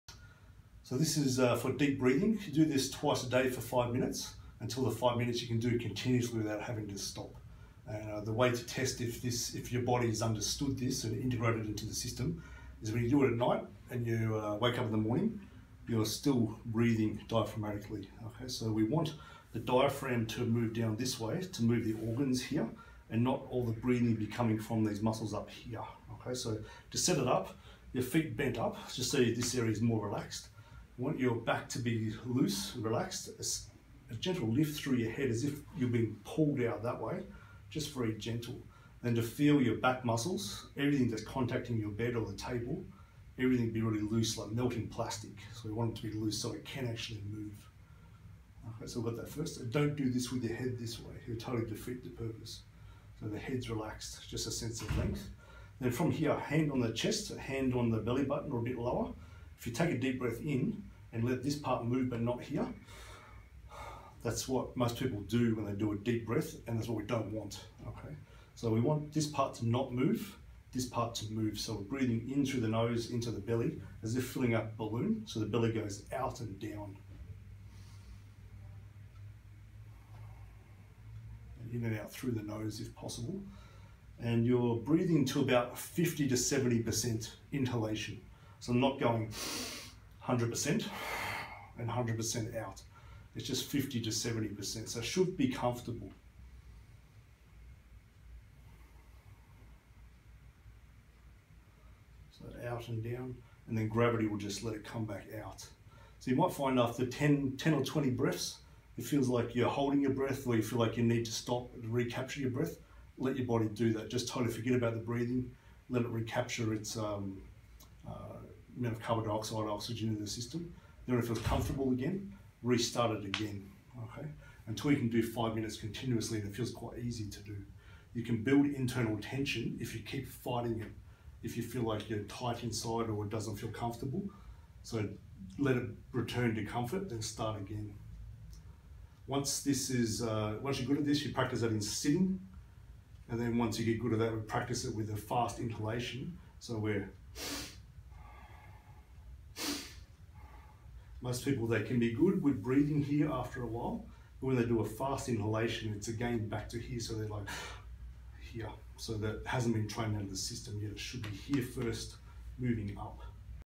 Restoring Deep Belly Breathing
2 times a day for 5 minutes.
Rest if feeling uncomfortable/under or over breathing feeling/ holding breath. Rest and let the body find its natural rhythm again.
Start in this position, then in sitting, standing, walking. This needs to become the normal way you breath.